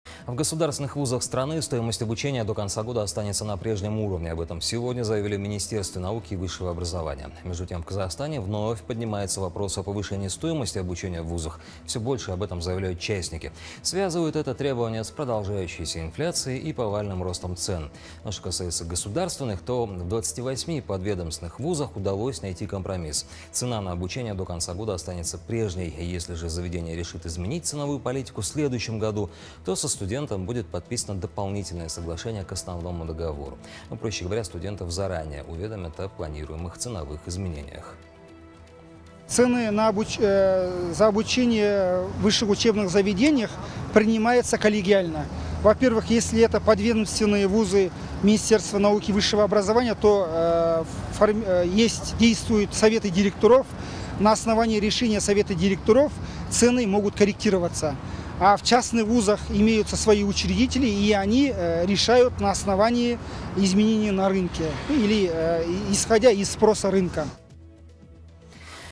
МНВО РК: Стоимость обучения в госвузах останется прежней
В государственных вузах страны стоимость обучения до конца года останется на прежнем уровне. Об этом сегодня заявили в Министерстве науки и высшего образования. Между тем, в Казахстане вновь поднимается вопрос о повышение стоимости обучения в ВУЗах. Все больше об этом заявляют частники. Связывают это требование с продолжающейся инфляцией и повальным ростом цен. Ну, а что касается государственных, то в 28 подведомственных вузах удалось найти компромисс. Цена на обучение до конца года останется прежней. Если же заведение решит изменить ценовую политику в следующем году, то со студентом будет подписано дополнительное соглашение к основному договору. Тем самым обучающийся будет заранее уведомлен о планируемых ценовых изменениях.